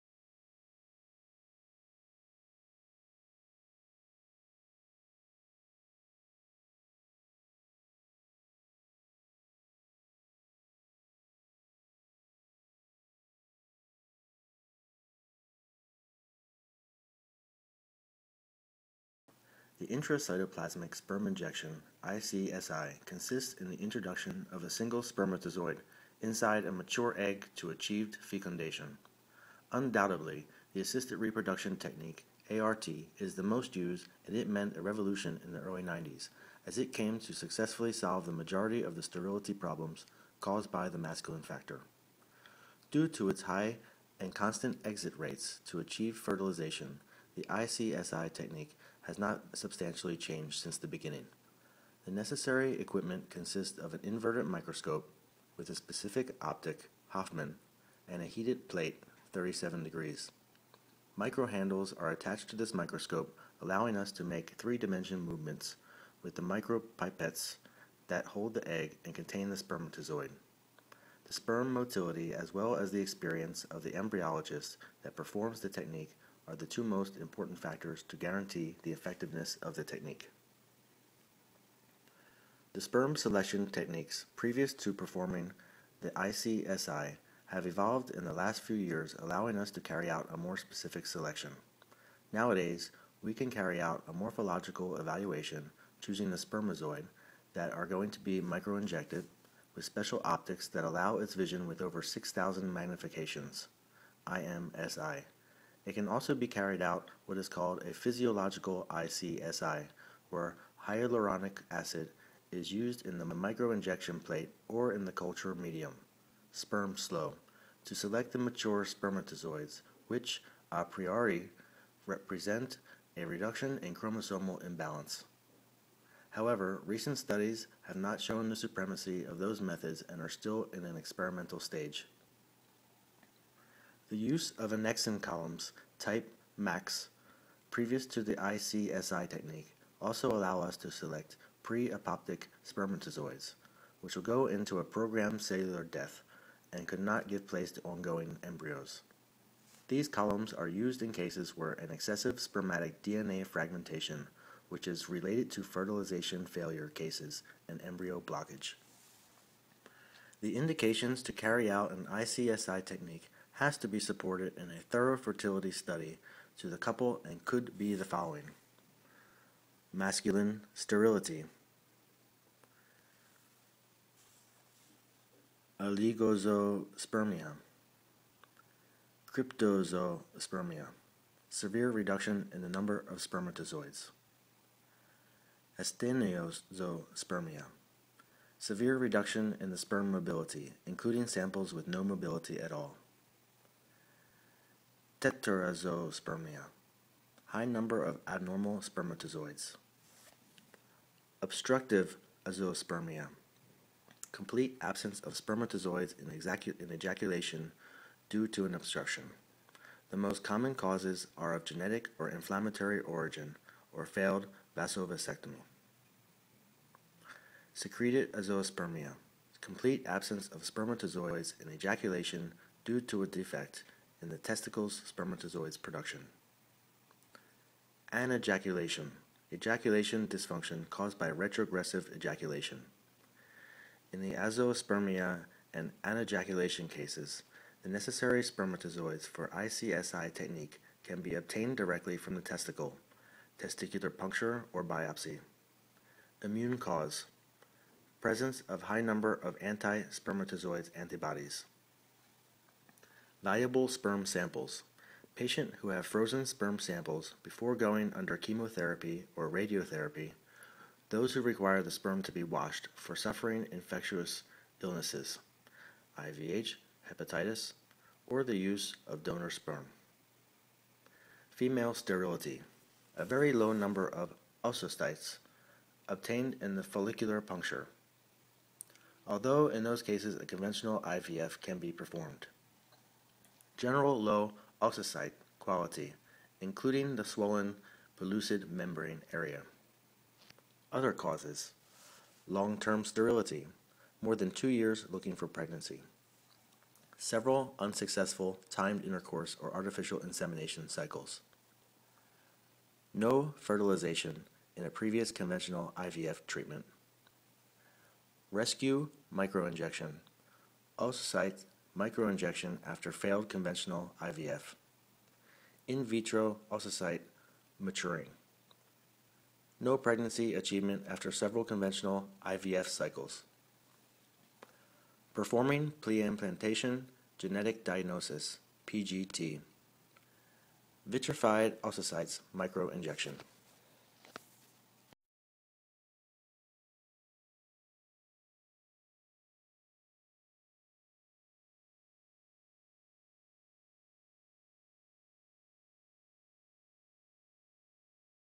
ICSI: Intracytoplasmic Sperm Injection
The Intracytoplasmic Sperm Injection (ICSI) consists in the introduction of a single spermatozoid inside a mature egg to achieved fecundation.
Due to its high and constant exit rates to achieved fertilization, the ICSI technique has not substantially changed since the beginning.
The indications to carry out an ICSI technique has to be supported in a thorough fertility study to the couple.